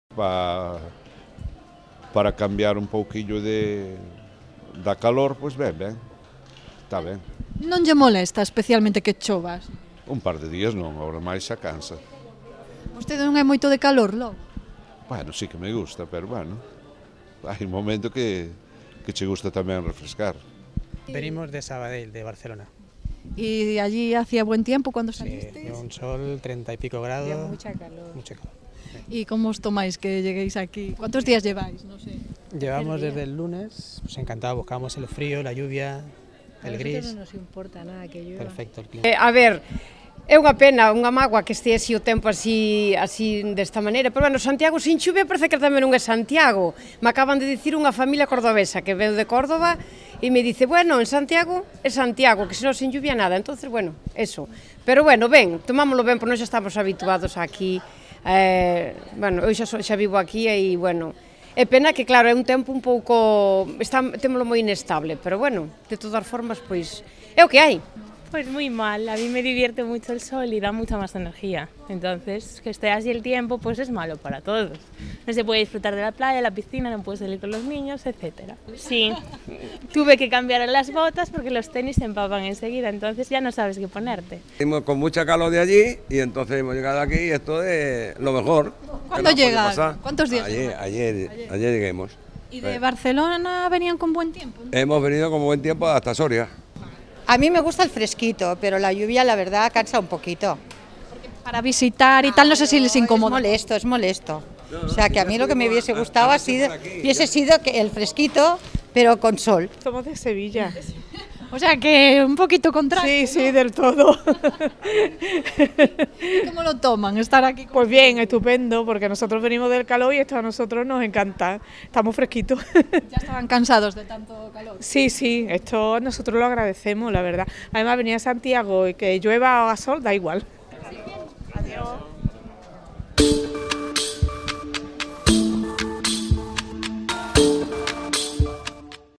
Ao mal tempo boa cara?
Como se toman  os cidadáns o tempo que está deixando agosto estes días en Galicia con chuvia e baixas temperaturas?